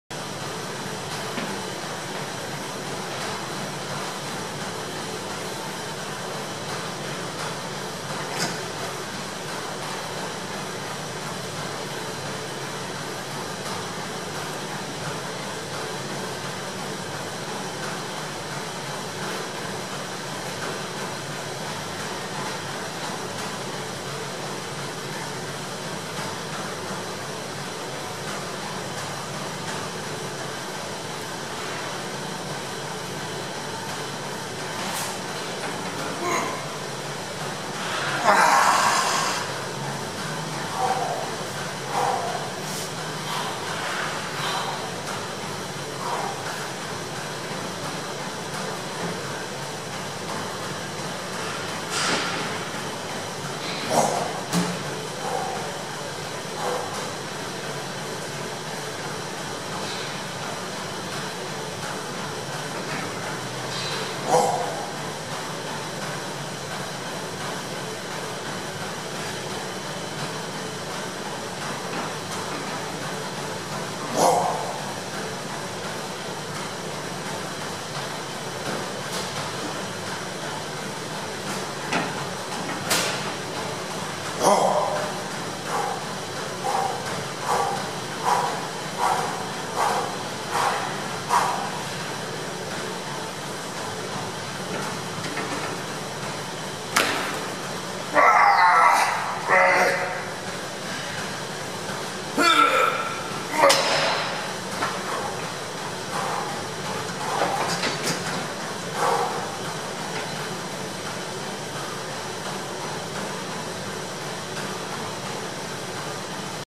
MEDX LEG PRESS. 2ND SET HEAVY LOW REPS.
After my high rep assault. I continued on by changing several variables. One I place the seat in full recline squat squat position, which ups the degree of difficulty considerably by taking out the leverage advantage of an upright seat. Two I increased my range of allowable motion by moving the seat several clicks closer to the platform. I kept the same full stack weight and I was thoroughly pre exhausted by the 1st assault on my quads. This set was brutal.